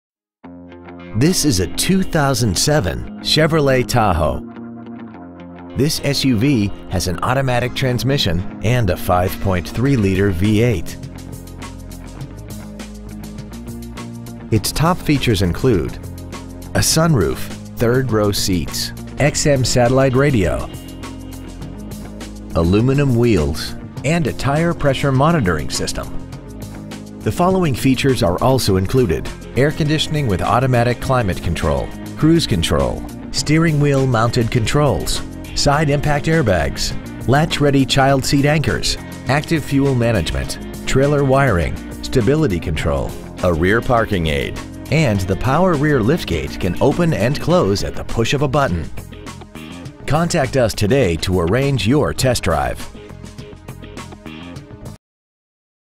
2007 Chevrolet Tahoe Dallas TX
Year : 2007
 Make : Chevrolet
 Model : Tahoe
 Engine : 5.3L Vortec 1000 V8 SFI Engine
 Trans . : Automatic
 Exterior : Unspecified
 Miles : 133,100
 Interior : Unspecified

 2007 Chevrolet Tahoe Dallas TX